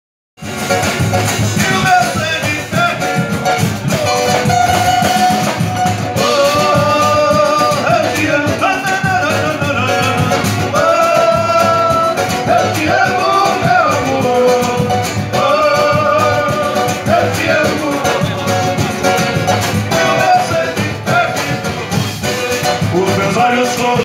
Ponto do Samba na Praça Nereu Ramos
Ponto do Samba, na Praça Nereu Ramos, contou neste sábado com a apresentação dos Amigos do Samba. Iniciativa da Fundação Cultural de Criciúma (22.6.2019)